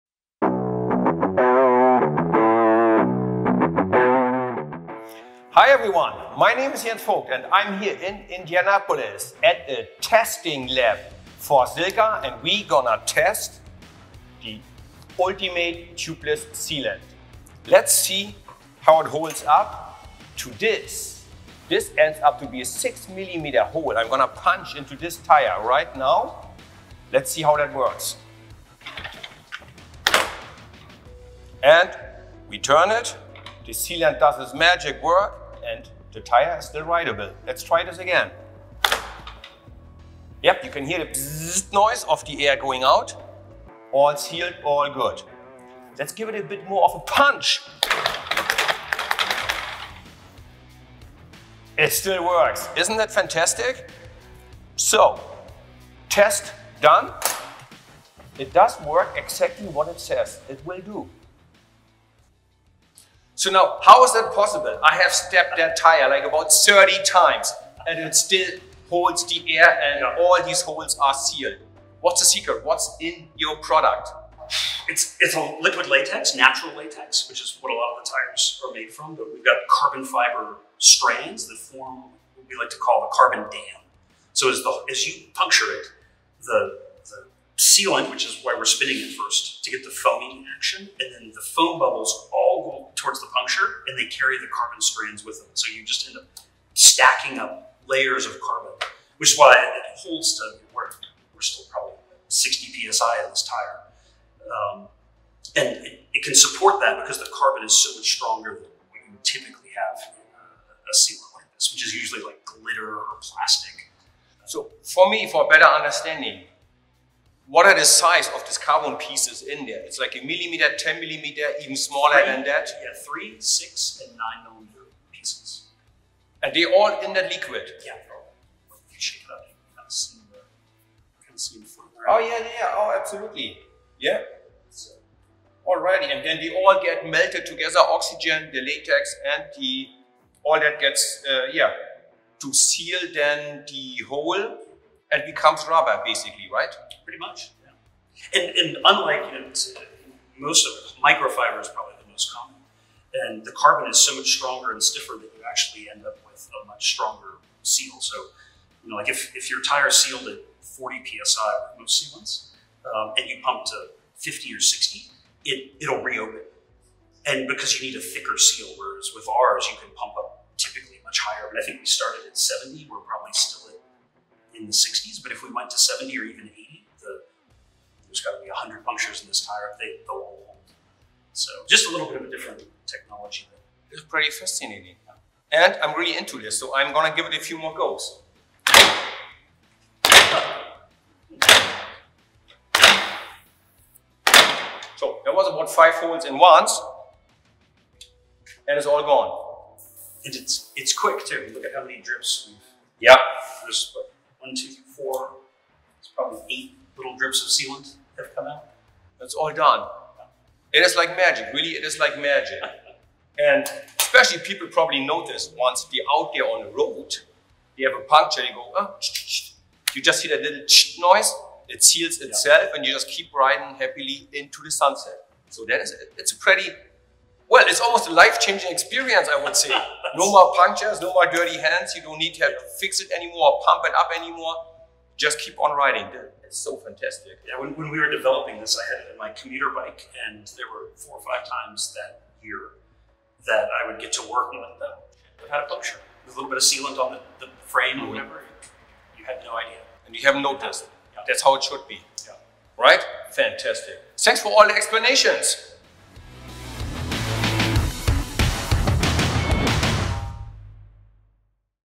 SILCA Test Lab with Jens Voigt EXTENDED Version
In this version, Jens tests SILCA's Ultimate Tubeless Sealant and asks Josh about the technology. Through the entire test, Jens stabs the tire 100+ times and it continues to hold. Learn about the tech behind the sealant and why it's so effective.